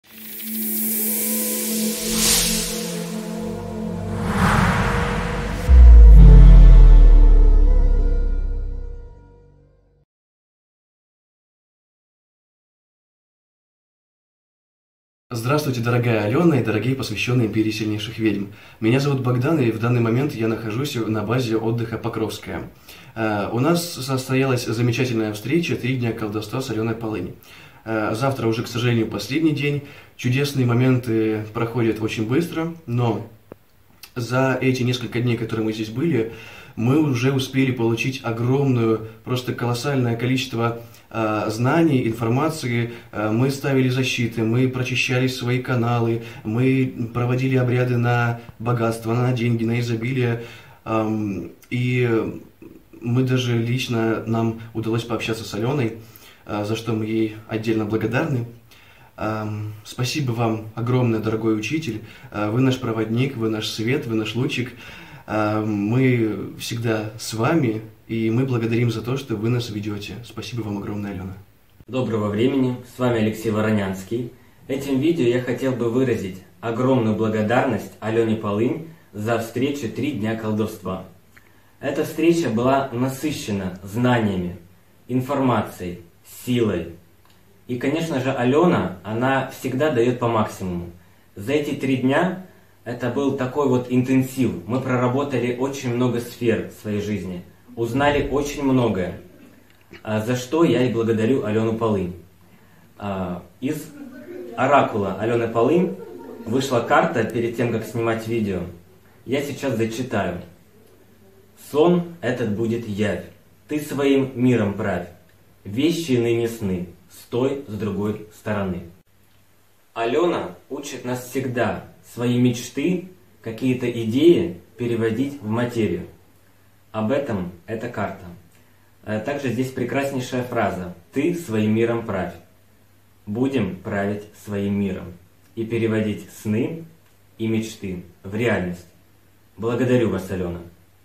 Благодарность Алене Полынь. Царский лес
Мы благодарим Алену Полынь за три чудесных дня в Царском лесу!

Подробности на авторском сайте ведьмы (Империя Сильнейших Ведьм)